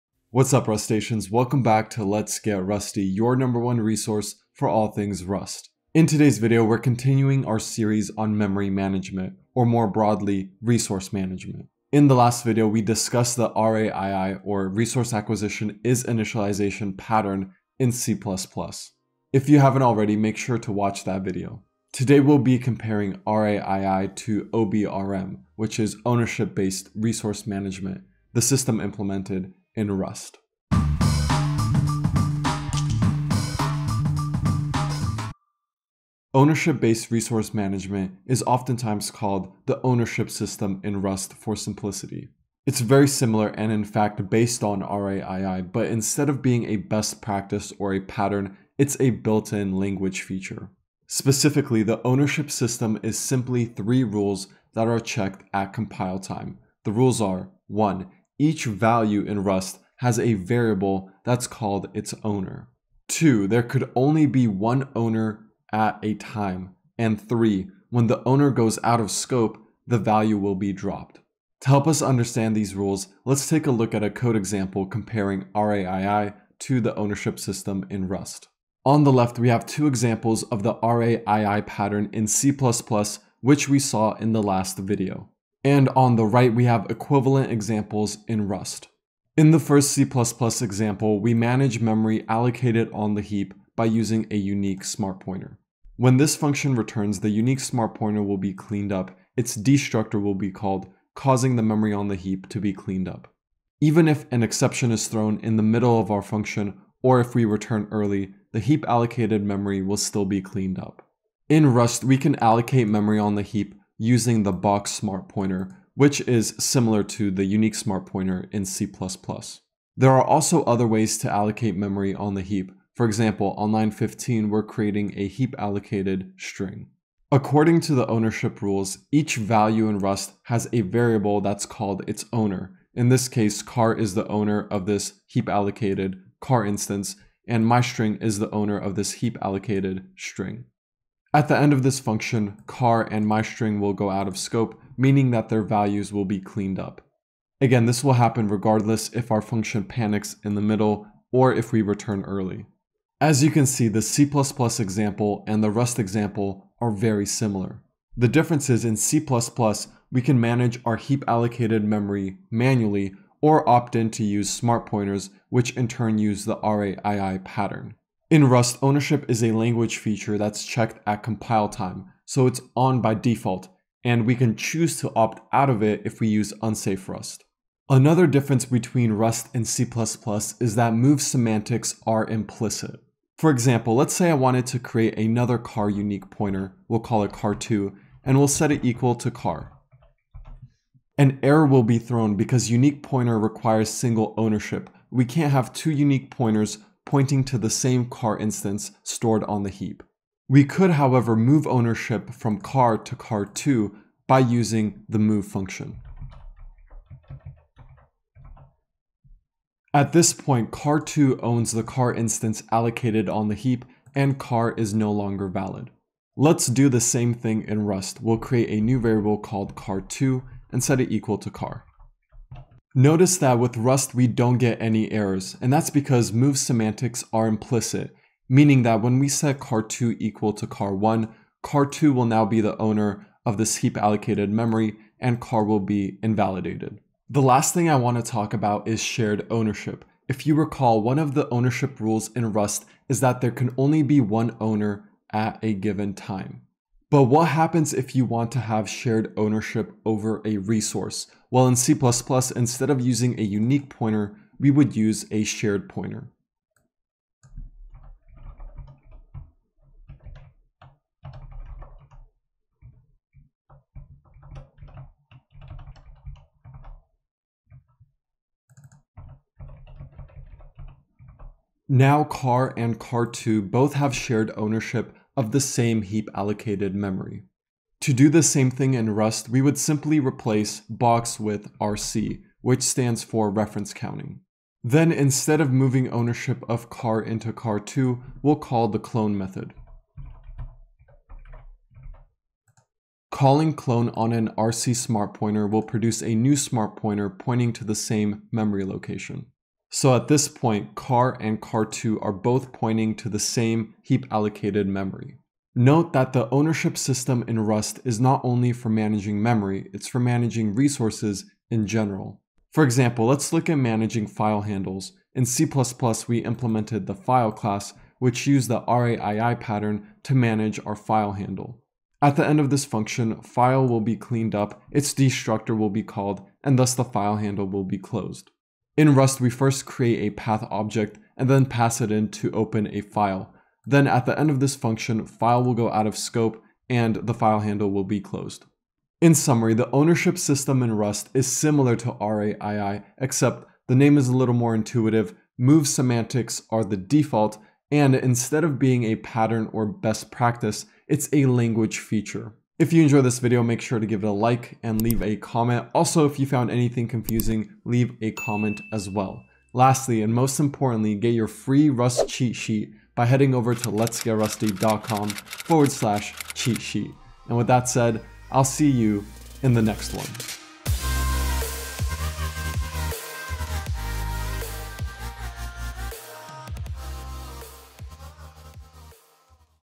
C++ RAII vs Rust OBRM - Part 2
· Today we will be discussing ownership in Rust and how it relates to the RAII pattern in C++.